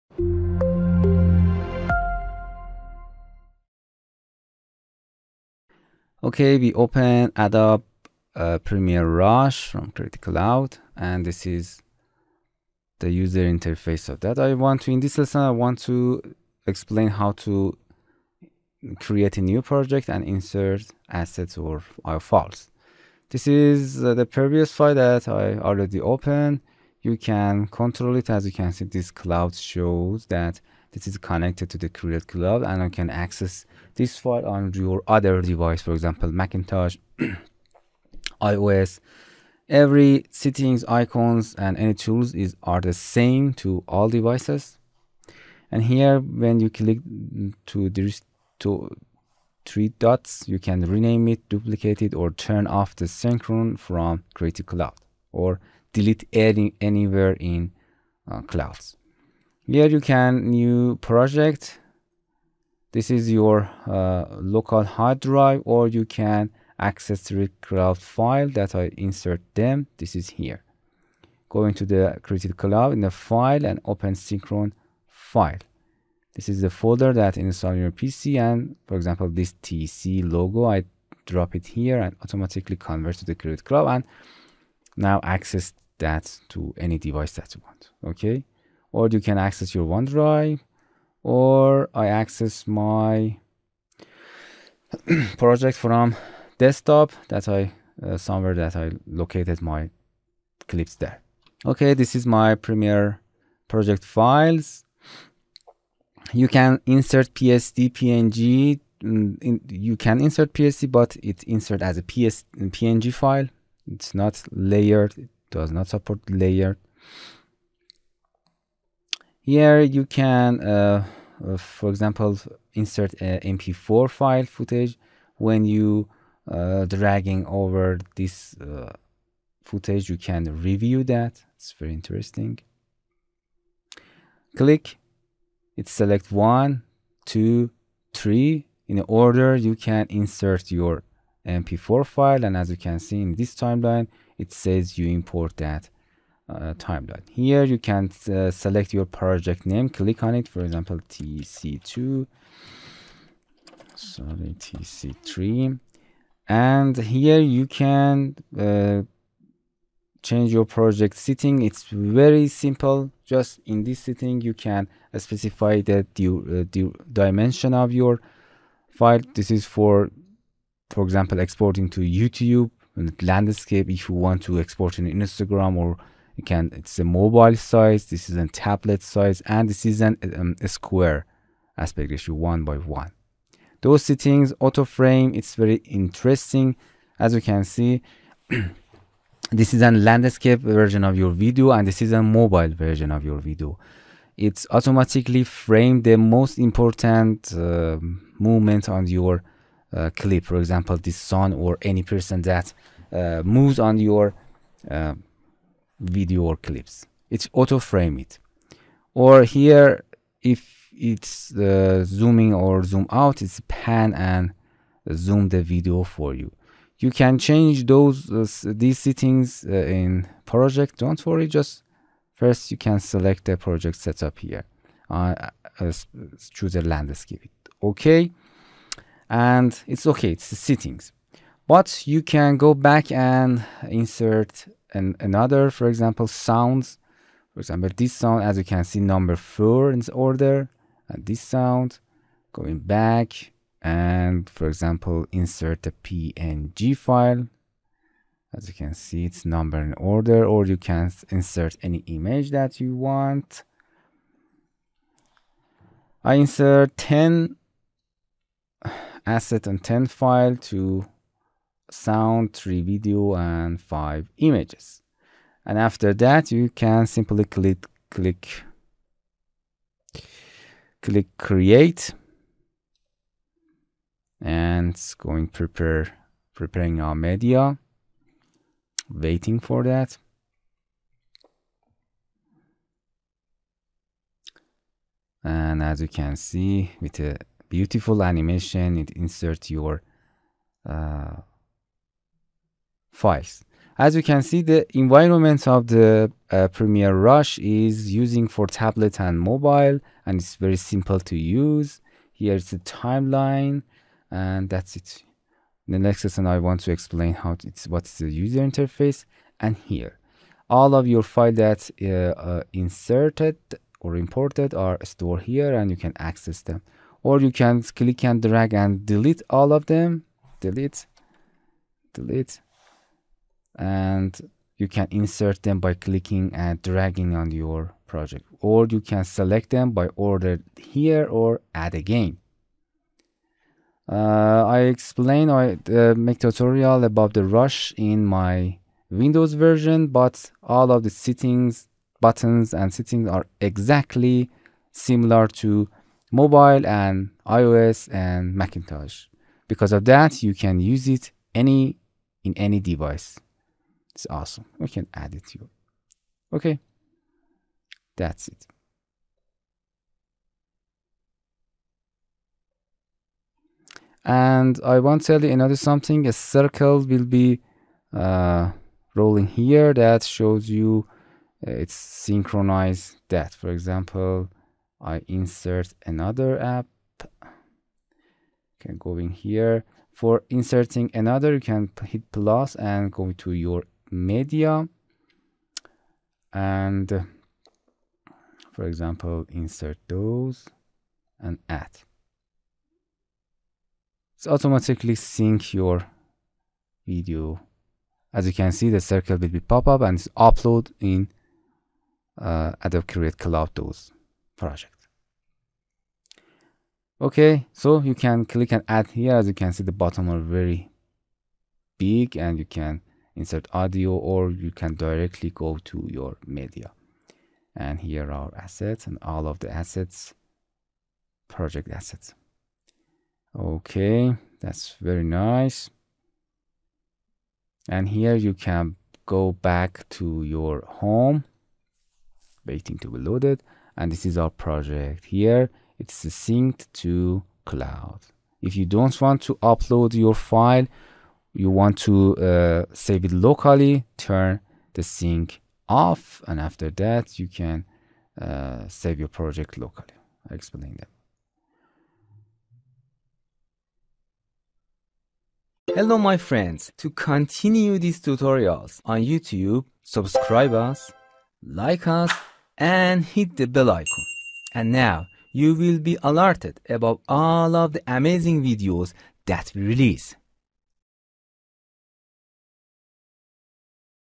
0001. new project & import files in premiere rush
In this lesson we want to explain how to import different types of files and create a new project.